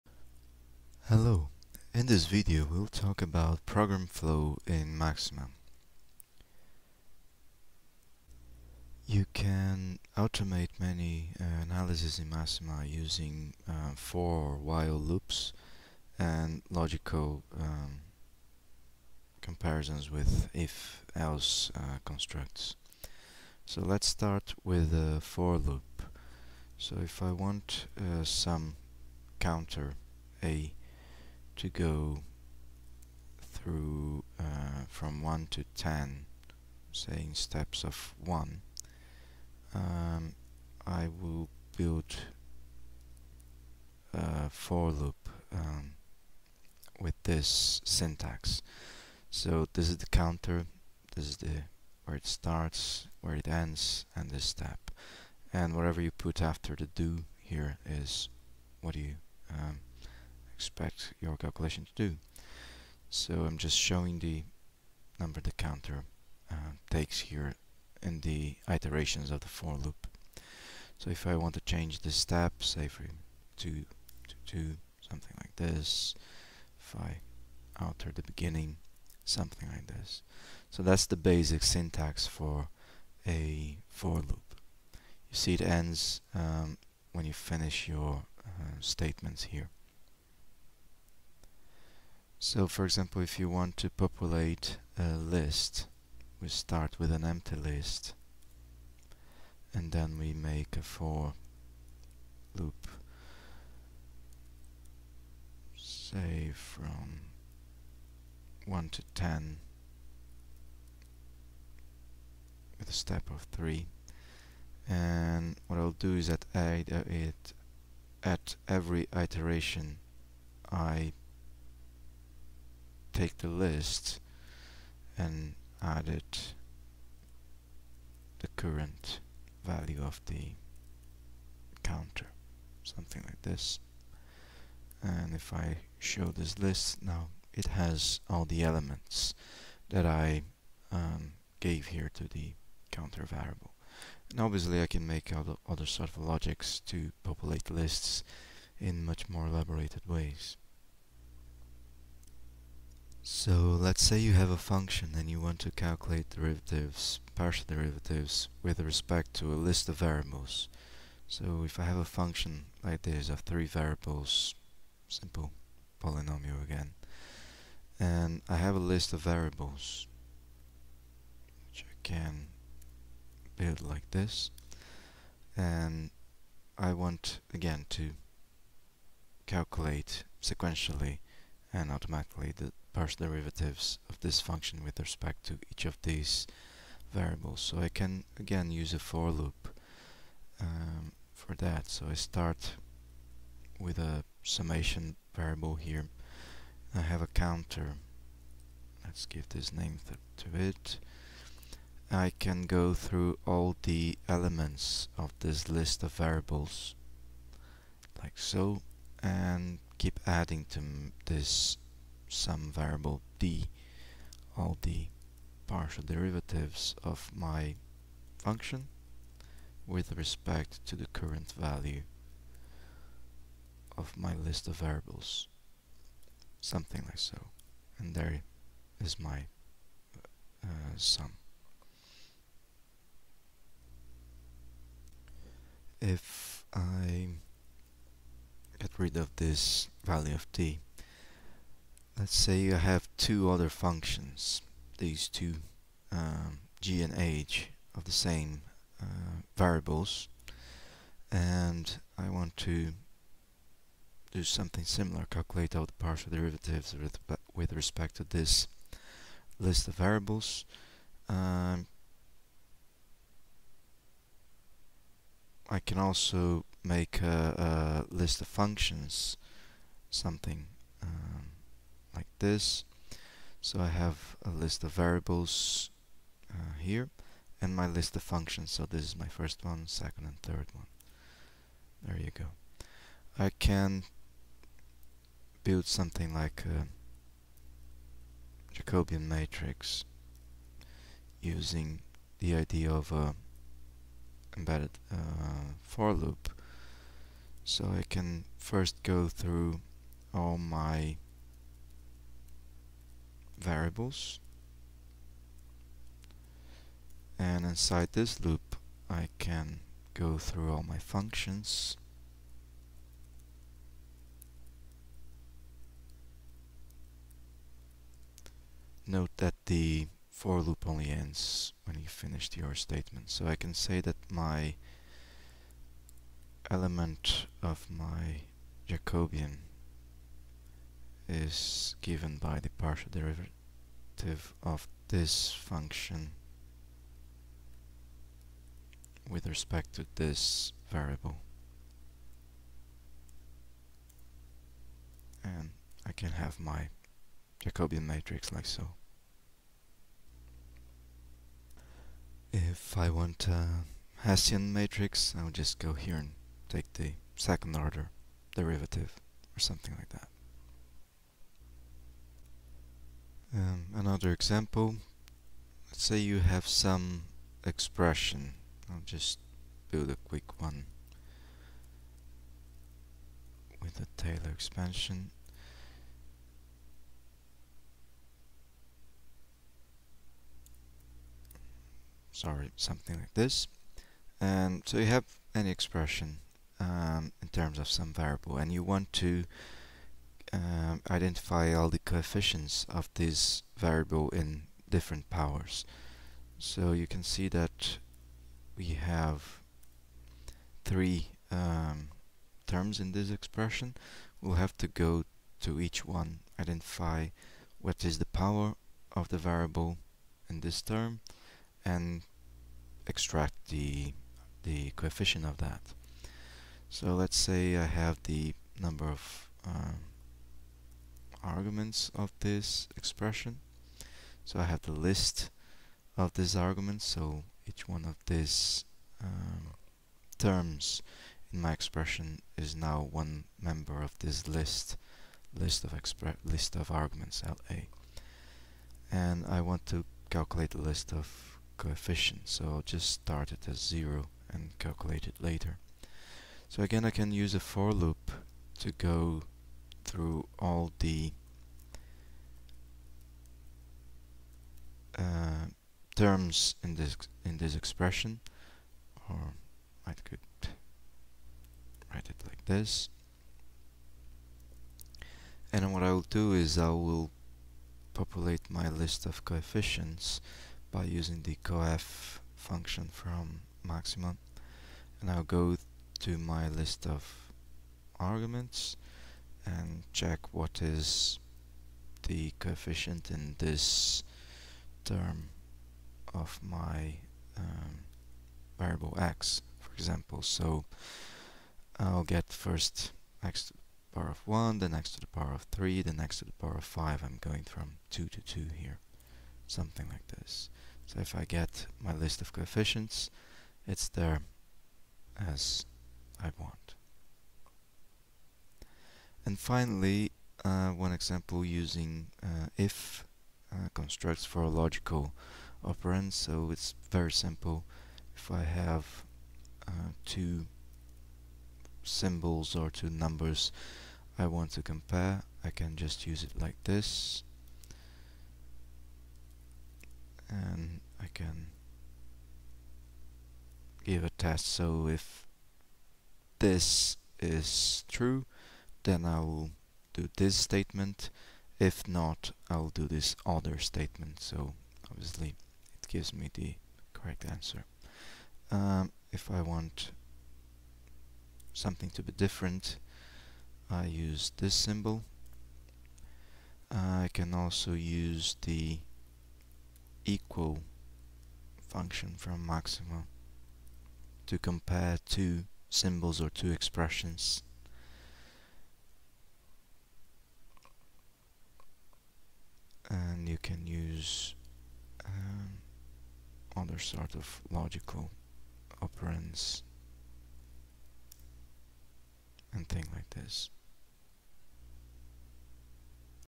maxima 07 program flow
introduction to using maxima for engineering problems
program flow, for loops and if statements